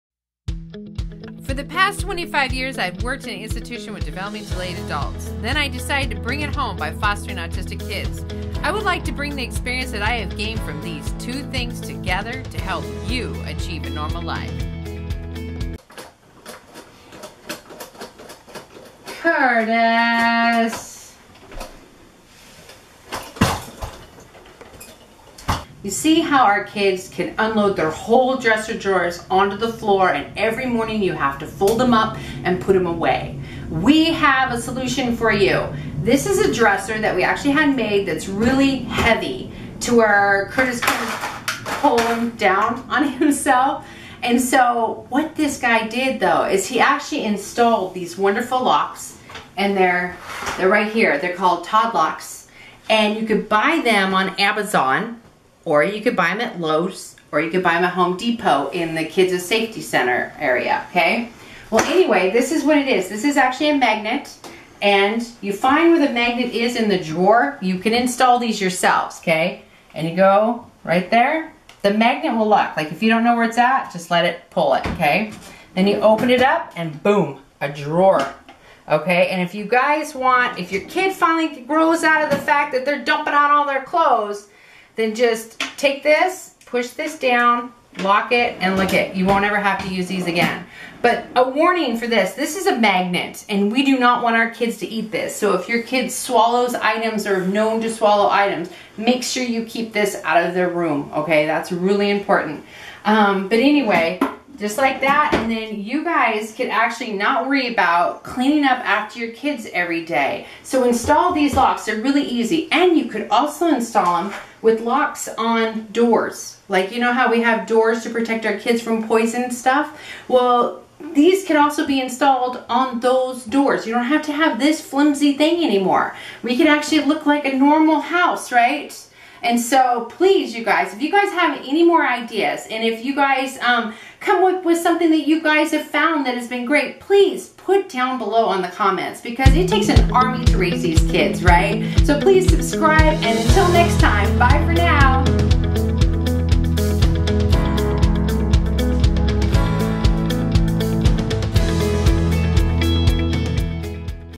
Stop Autistic Kids from getting into their drawers!
Autistic behavior

One behavior I have noticed in many special needs children, is a preoccupation with clothes items.

Some may enter into the rooms of others and steal their clothes, or empty out their dressers in order to put on as many clothing items as physically possible. As a mother this could become very exhausting to find your child’s clothes all over their floor, or find your child dressed in four sets of clothes. If your child isn't toilet trained, or if they are incontinent you may feel the need to re-wash every single piece of clothing outside of the drawers, whether they seem dirty or not. This consumes a lot of time and energy, wouldn't it be nice if we could prevent this completely?

This is why we want to show you an easy way to adapt your child’s dresser to increase your child’s safety, and to give your family a little more normalcy in life.

Our quick fix can be found at Lowes. They are called “Tot Loks” by “Safety 1st”. Make sure before you install these locks, you unscrew or otherwise remove the handles that are already on your dresser. This makes it more safe so that your child does not pull on the locked handles and bring their dresser down on themselves!

The locks and key go for about $20.00. You can watch our youtube video here to see how we install them. Please leave any ideas and comments on the video. We love hearing your insight, it takes an army to raise an autistic child! out this great idea from the man who designed Curtis dresser. The locks come with direction.  Tot lock is the brand order from amazon, home depot or lows stores.

The instructions are easy and the here is the youtube on how to apply the lock.